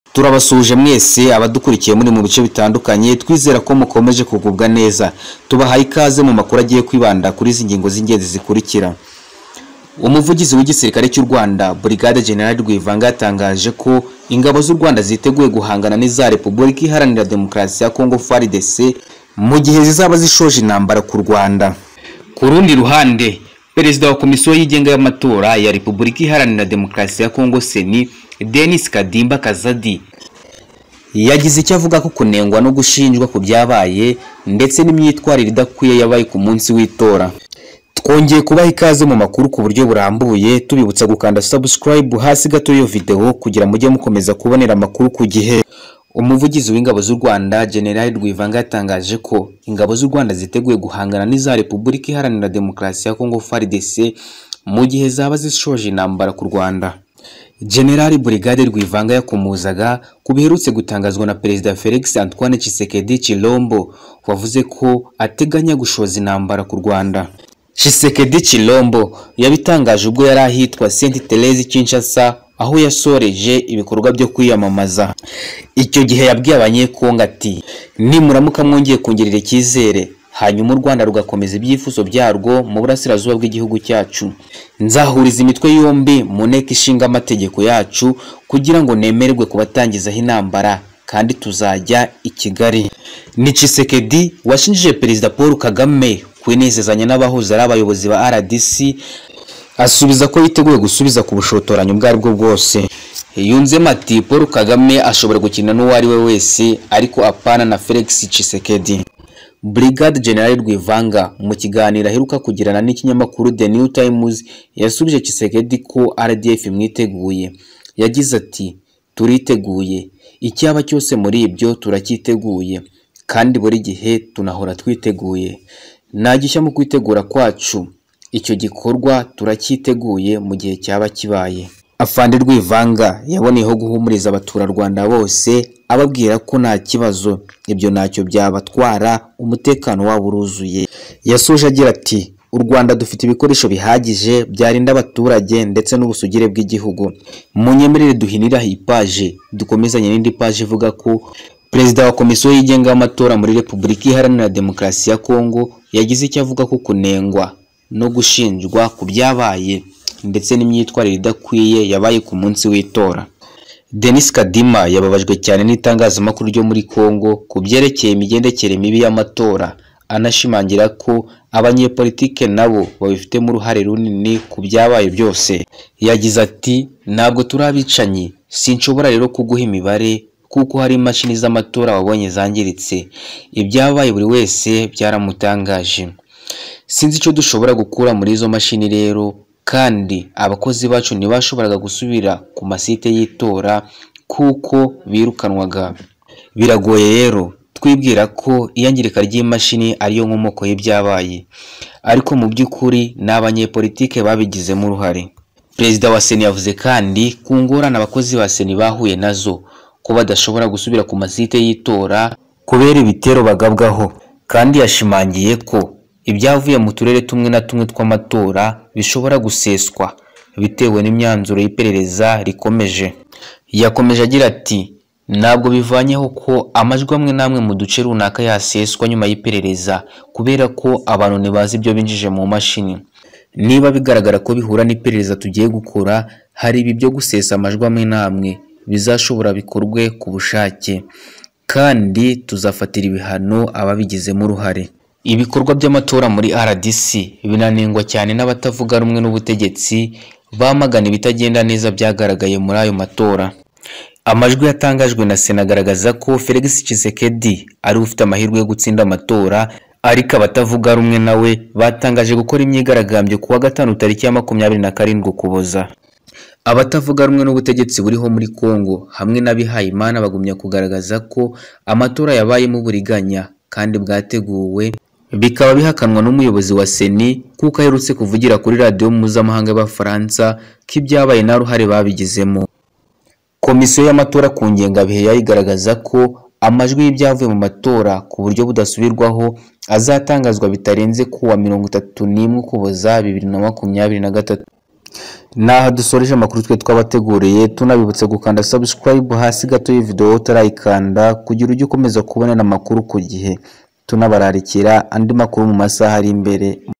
RWANDA(RDF),CONGO(FARDC).🚨URWANDA RWONGEYE GUHA CONGO UBUTUMWA BWANYUMA BWO KUYIBURIRA .
Umuvugizi w’Igisirikare cy’u Rwanda Brig Gen Ronald Rwivanga yatangaje ko igisirikare cya urwanda kiteguye guhangana nigisirikare cya fardc igihe cyose kizashoza intambara kurwanda.